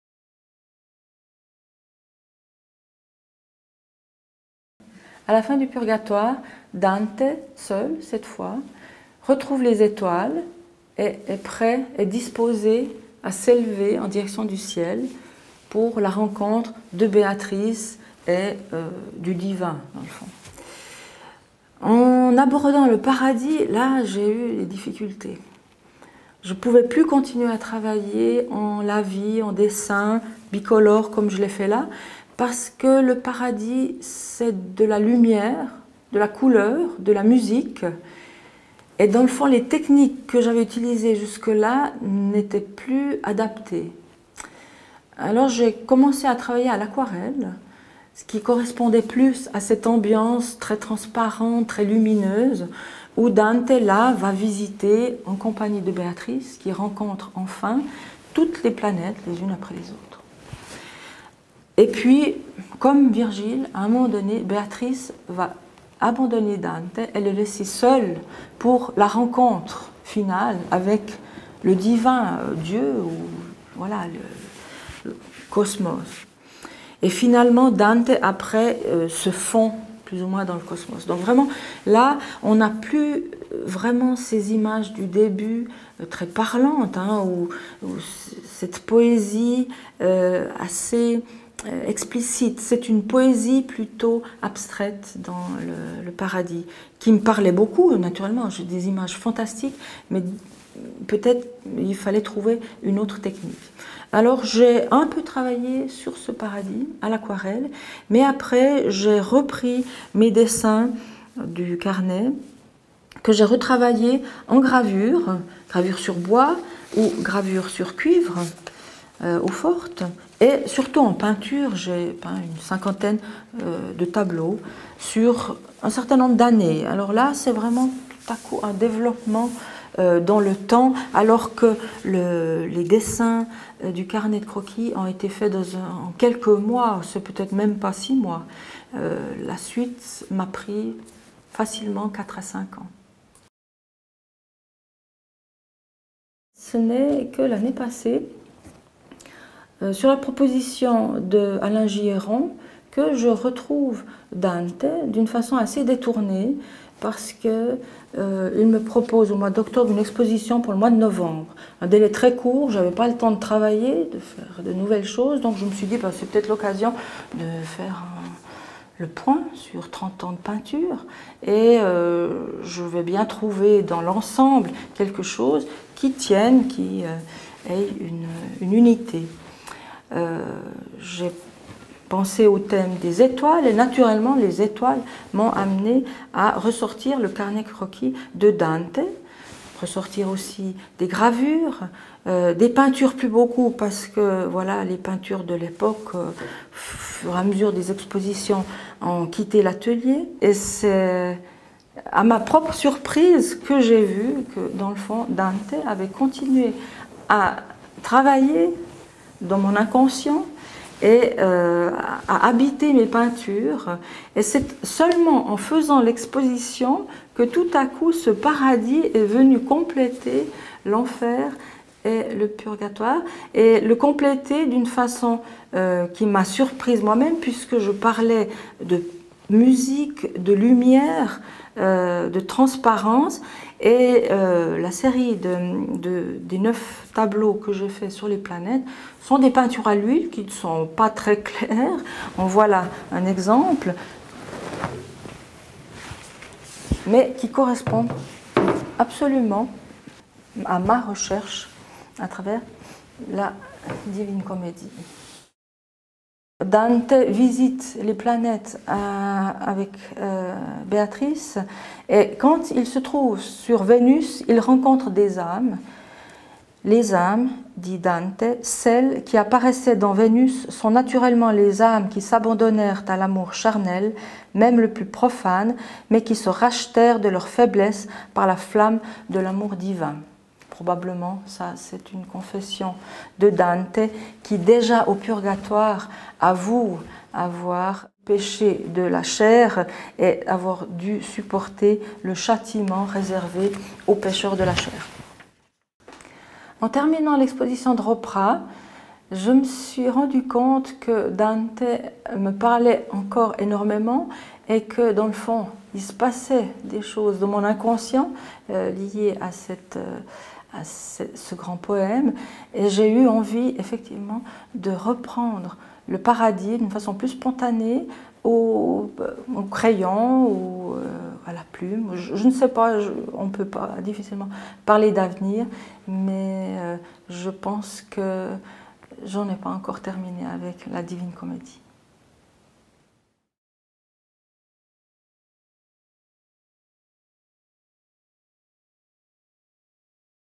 Le paradis de Dante par Claire Nydegger
Interview filmée de Claire Nydegger, le 14 mai 2014 à Saint-Prex. Réalisation, prise de vues et montage : Laurent Dubois.